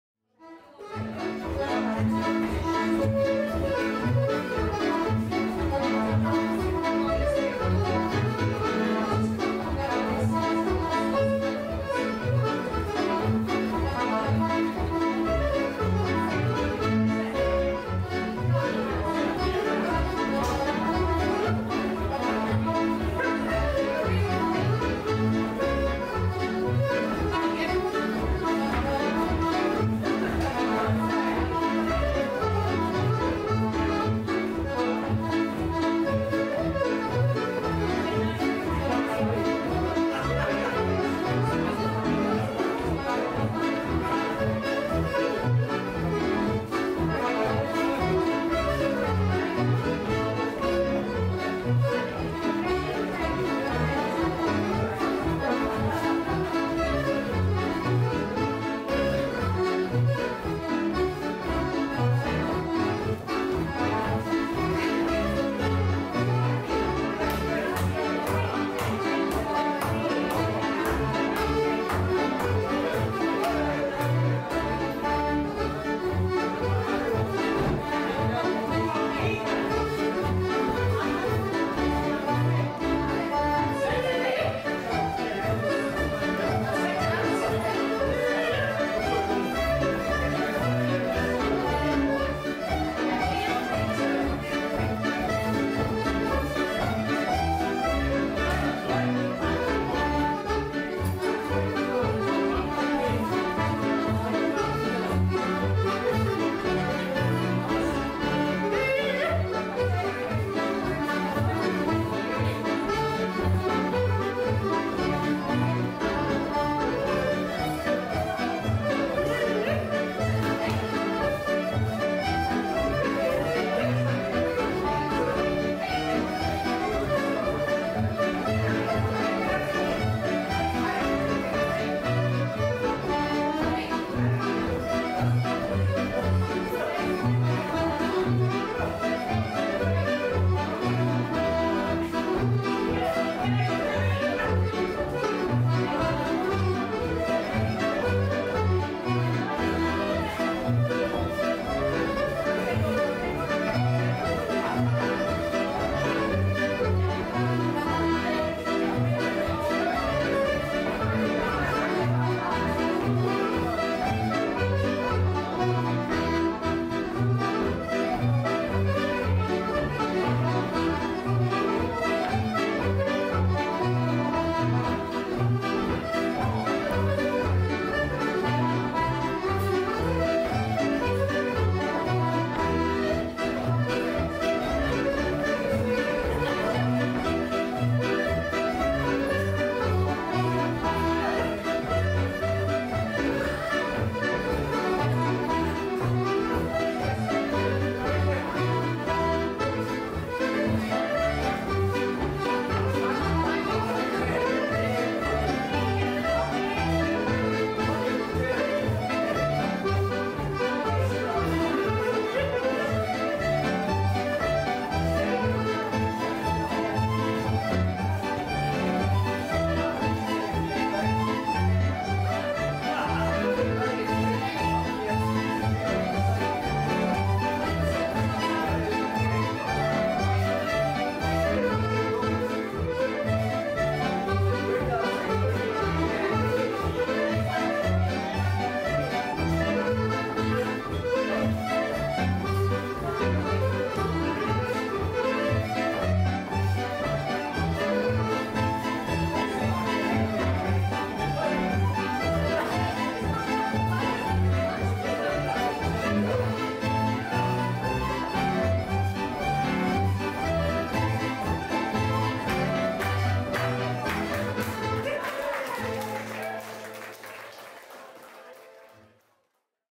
Broons Reel
A lively version of this dance no stuffed shirts here just as Broons Reel should be danced. This brings back a lot of happy memories for me nae pointy toes in this version just nice and lively..